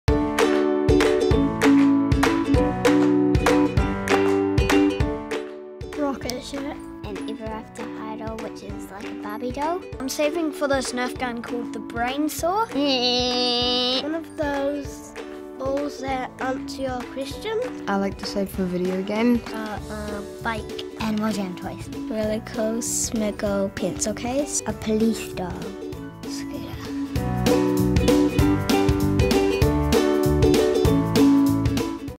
We ask WKids Club members whether they're saving for anything special
Hear what some of the Westfield Kids Club members are saving their hard earned pocket money for.